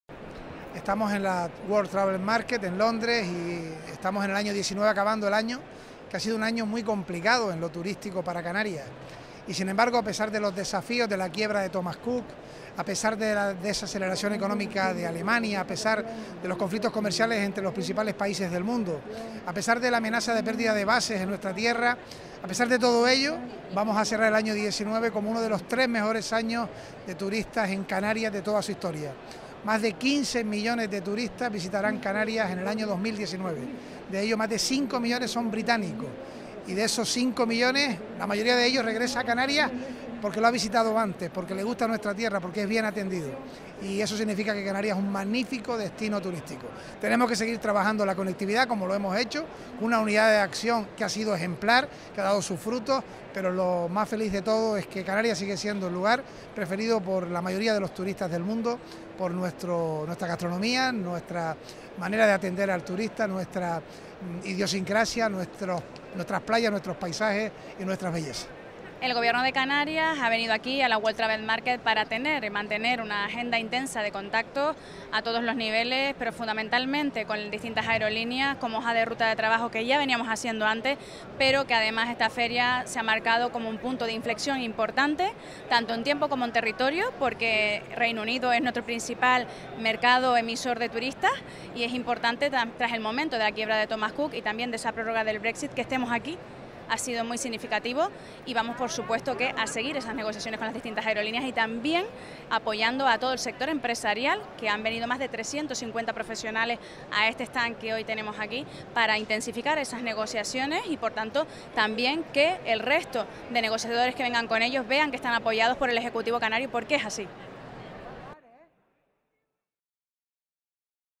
Canarias en la World Travel Market de Londres 2019
El presidente Ángel Víctor Torres anuncia en la inauguración de la World Travel Market de Londres que Canarias cerrará 2019 con algo más de 15 millones de turistas.

Fecha: 4 de noviembre de 2019
Lugar: Londres
Créditos: Presidencia del Gobierno